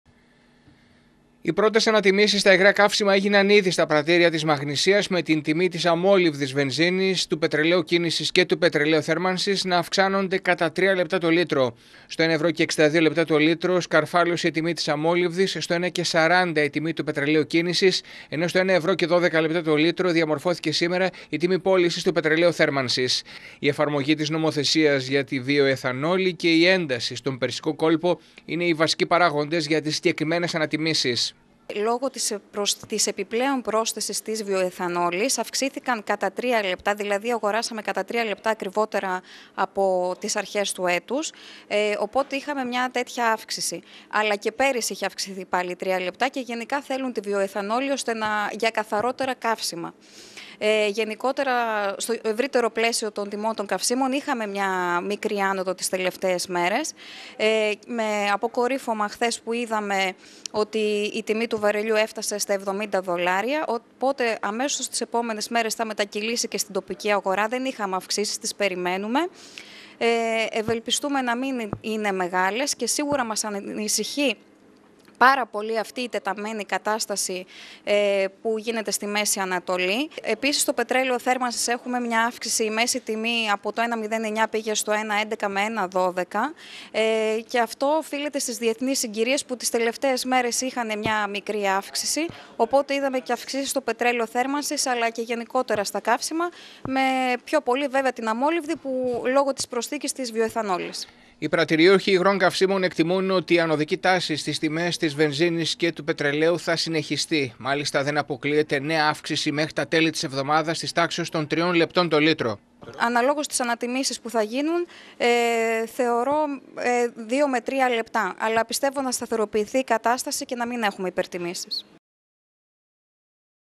Βόλος Με το «καλημέρα» του νέου χρόνου, αντιμέτωποι με ανατιμήσεις στην αγορά καυσίμων 080120
Λίγες ημέρες μετά την έλευση του νέου χρόνου οι καταναλωτές και στη Μαγνησία βρίσκονται αντιμέτωποι με ανατιμήσεις στην αγορά των καυσίμων. Τόσο η αμόλυβδη βενζίνη και το πετρέλαιο κίνησης, όσο και το πετρέλαιο θέρμανσης ακρίβυναν κατά τρία λεπτά το λίτρο. Στα καύσιμα κίνησης οι ανατιμήσεις προκλήθηκαν από την υποχρεωτική αύξηση του ποσοστού πρόσμιξης της βιοαιθανόλης στην αμόλυβδη και το πετρέλαιο. Για την αύξηση στο πετρέλαιο θέρμανσης δεν υπάρχει σαφής εξήγηση, ενώ η μέση τιμή του στη Μαγνησία διαμορφώθηκε σήμερα στο 1 ευρώ και 12 λεπτά το λίτρο.